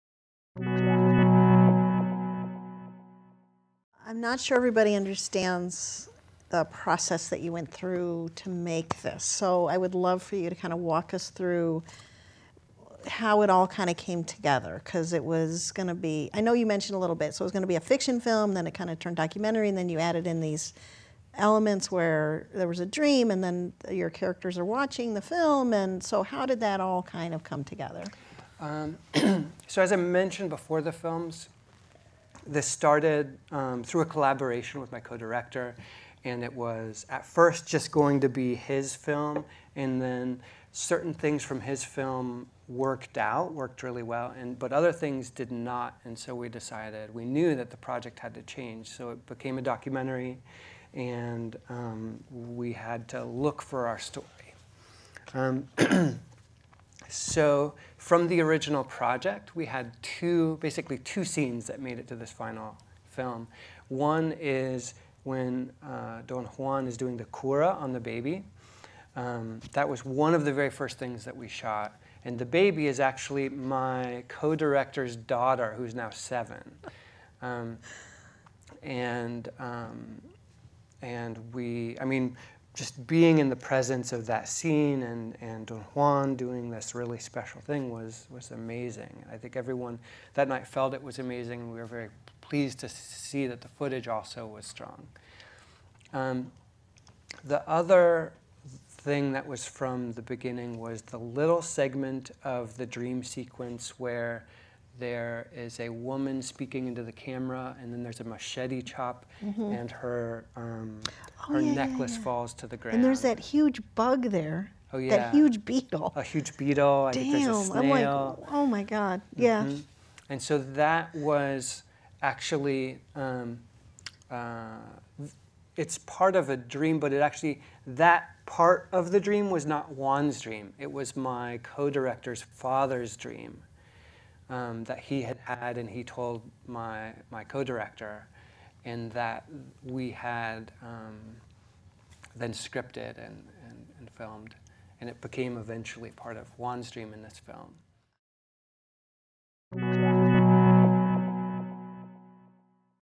Making The Modern Jungle with Charles Fairbanks, Director, The Modern Jungle (2/3) | DePaul VAS
Charles Fairbanks grew up in Nebraska and wrestled at Stanford, where he studied Art and the History of Science. He has a MFA in Art & Design from the University of Michigan, where he also studied History and Anthropology. His films have shown on POV and at Anthology Film Archives, Slamdance, CPH:DOX, Visions du Réel, Art of the Real, and over 100 other festivals across five continents. Fairbanks has received fellowships from the Guggenheim Foundation, the MacDowell Colony, the Wexner Art Center’s Film/Video Studio Program, and the Ohio Arts Council. His first feature film The Modern Jungle was made in collaboration with Saul Kak, an ethnically Zoque painter from rural Chiapas, Mexico. The Modern Jungle won jury awards at Slamdance and at the Athens Film & Video Festival; it is distributed in North America by The Cinema Guild. Fairbanks is also a published photographer (Fortune Magazine) and writer (Senses of Cinema). Fluent in French and Spanish, he has worked as a film programmer and translator in Belgium, a professor of VideoArte in Mexico. Fairbanks founded the Media Arts program at Antioch College where he teaches documentary history and production; he also teaches cel-phone videography to young indigenous women activists in Oaxaca, Mexico.